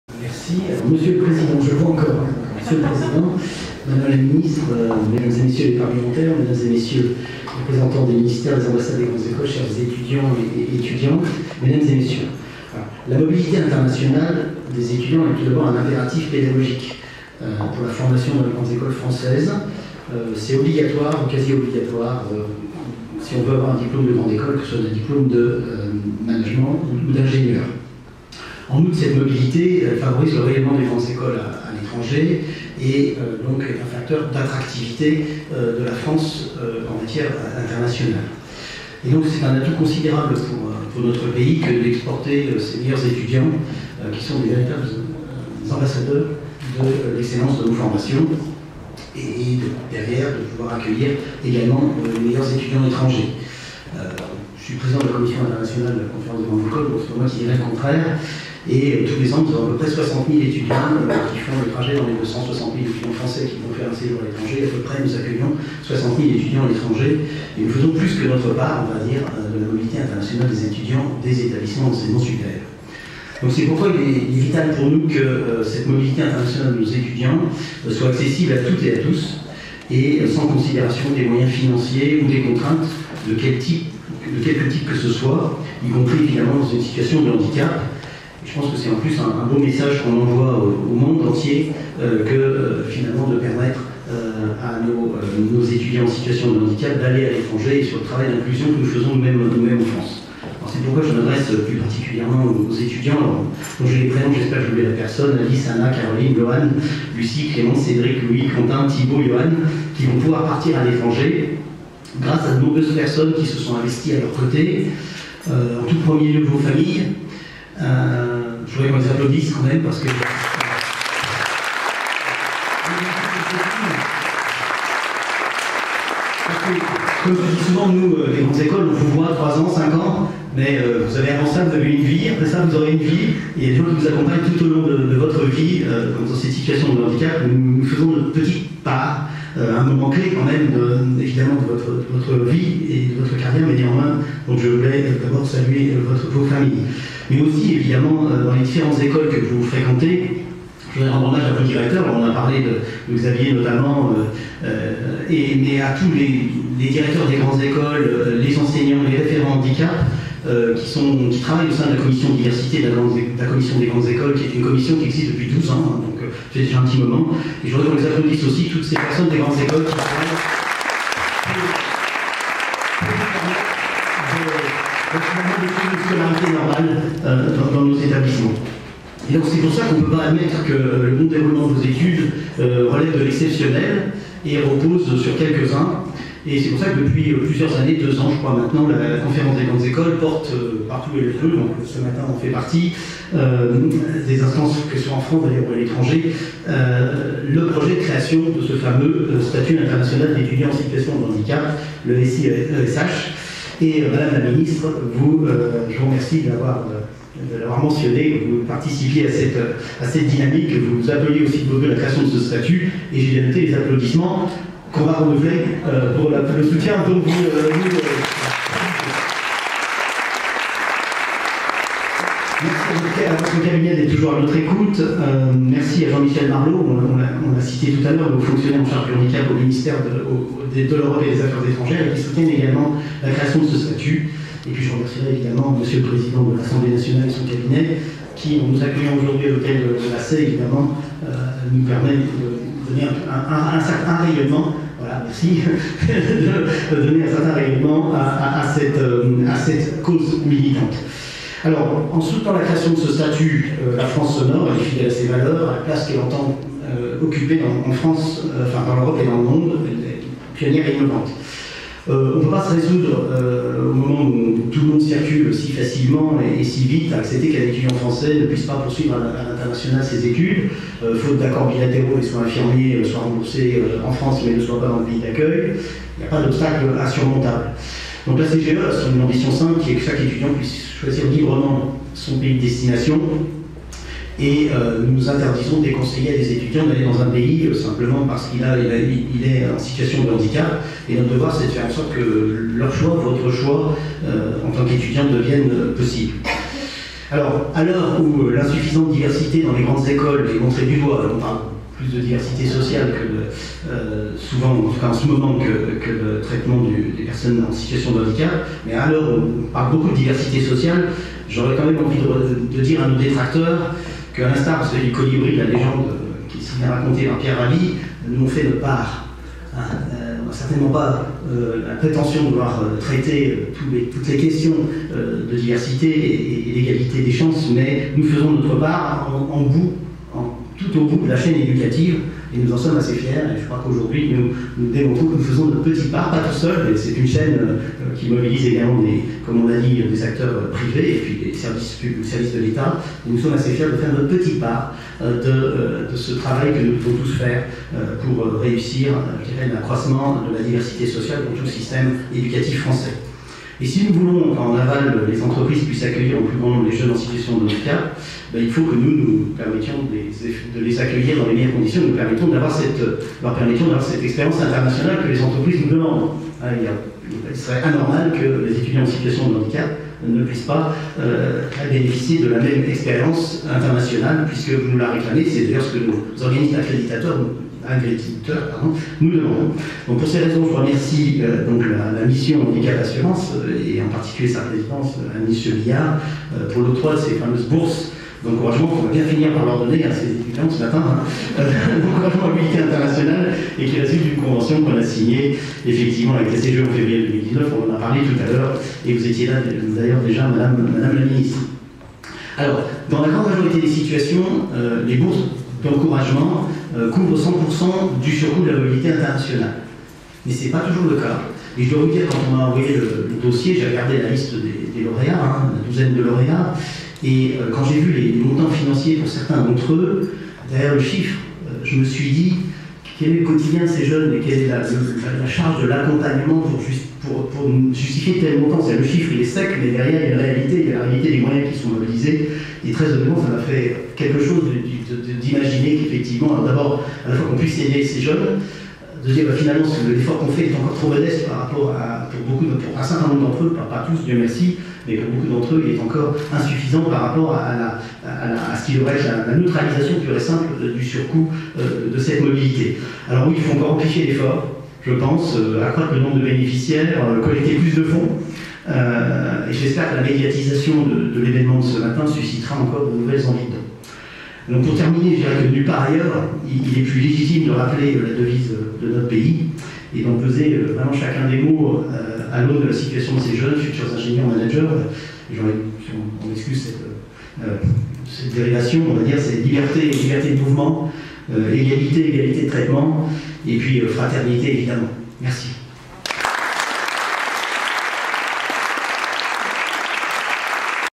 Bourses mobilité handicap : Y. Poilane, pdt de la commission Relations internationales de la CGE
Allocution de Yves Poilane, président de la commission Relations internationales de la CGE et directeur de Télécom Paris, le 4 juillet 2019  à l'Assemblée nationale, lors de la cérémonie de remise des bourses d’encouragement à la mobilité internationale des étudiants en situation de handicap.

Cet événement est sous le Haut Patronage de Monsieur Richard Ferrand, président de l’Assemblée nationale et avec le soutien de la Mission Handicap Assurance.

La Conférence des grandes écoles porte depuis plusieurs années le projet de création du Statut International d’Étudiant en Situation de Handicap (SIESH).

En attendant l’adoption de ce statut, qui a été présenté par la CGE aux plus hautes instances nationales et internationales et qui faciliterait la mobilité des étudiants en situation de handicap, l’octroi de ces bourses viendra alléger les surcoûts supportés par les étudiants et leur famille.